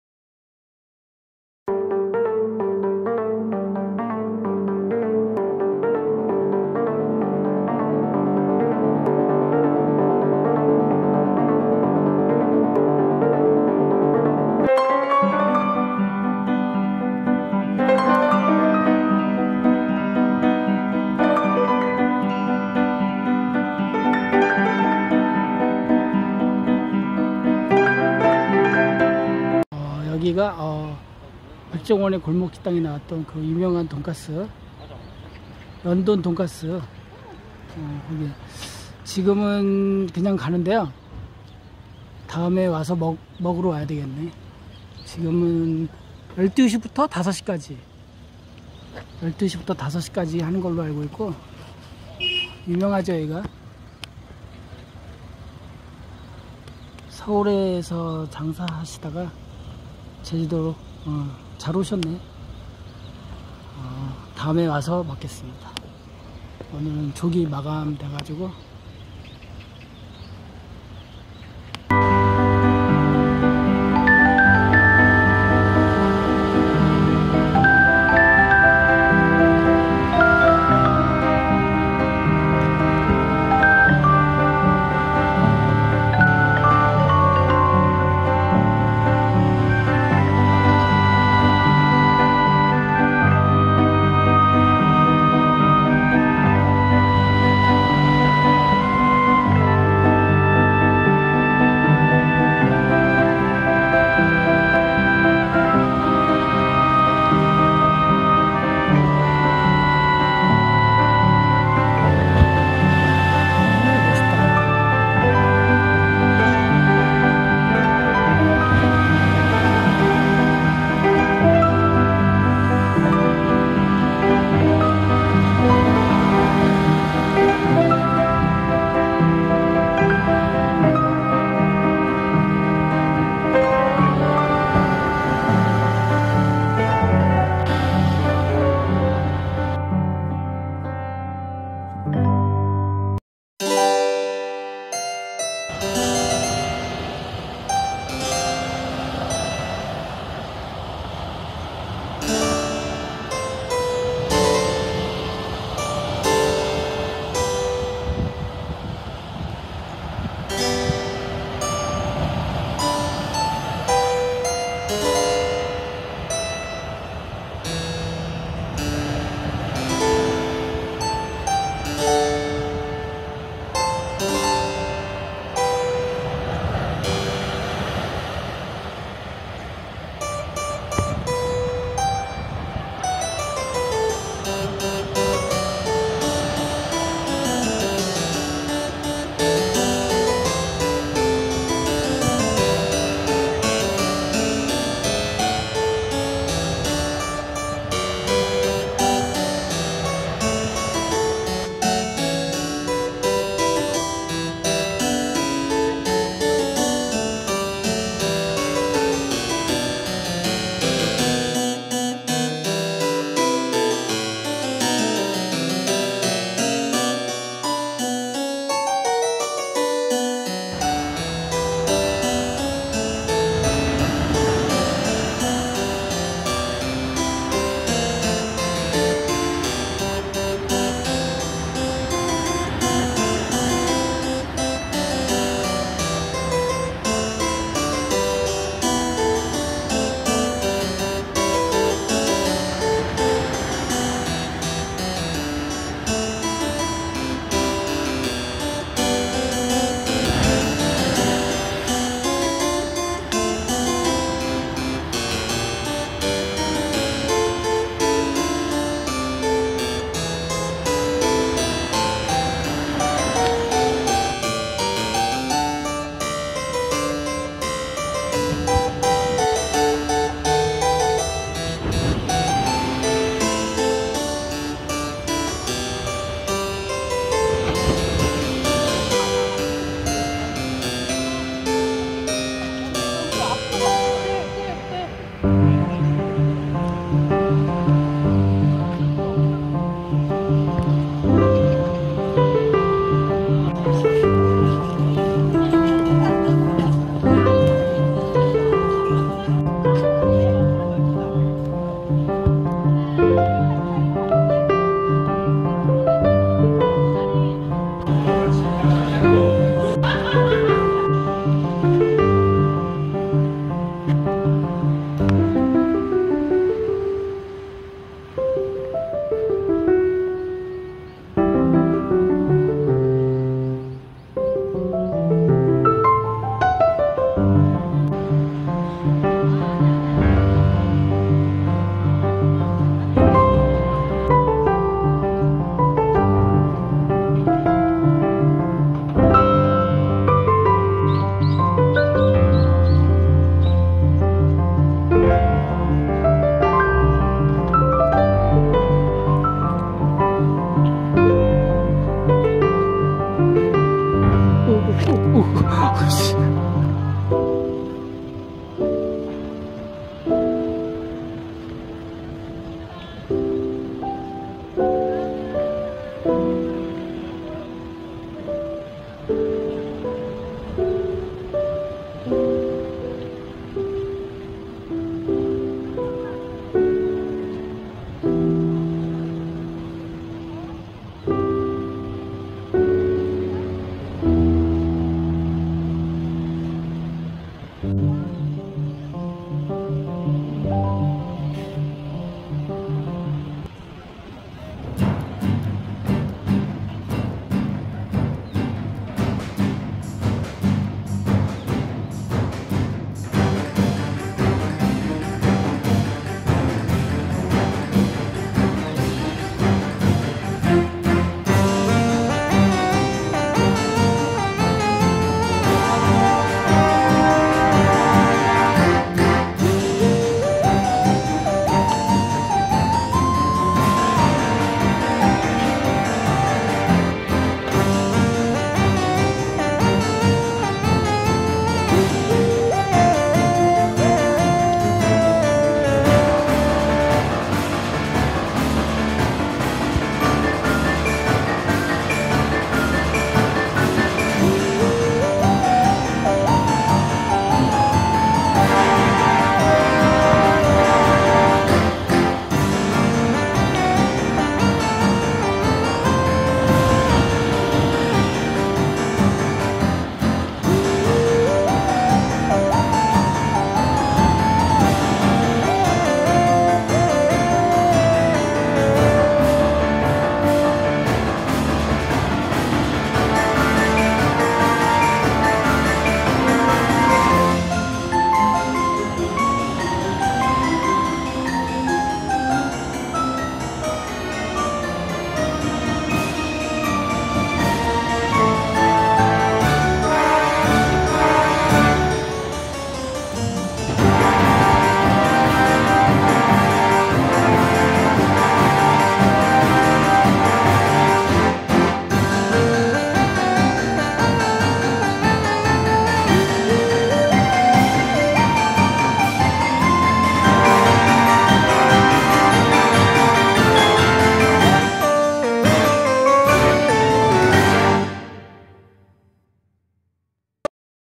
연돈돈까스는 다음기회에?
오늘 우리 집에서 12키로 거리 인테넷으로 백종원에 골목식당 소개 되었다 는 소문 난
연돈 돈까스 집 위치 파학 만 하고 그냥 집으로 오다 바다 근처 카페
촬영 하고 왔습니다